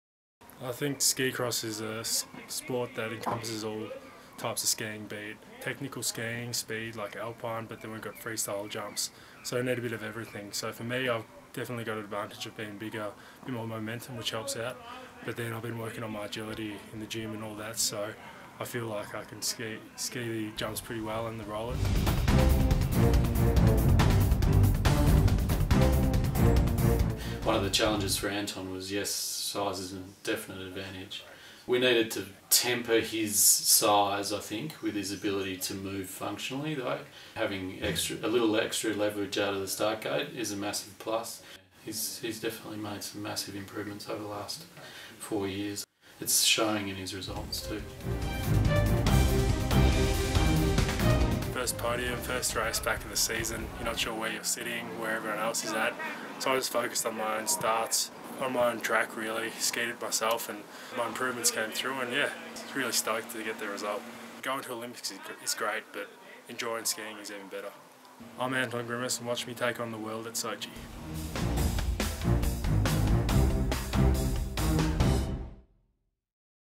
Meet Sochi2014 Winter Olympian Anton Grimus
NSWIS Ski Cross athlete Anton Grimus debuts at the Sochi 2014 Winter Olympics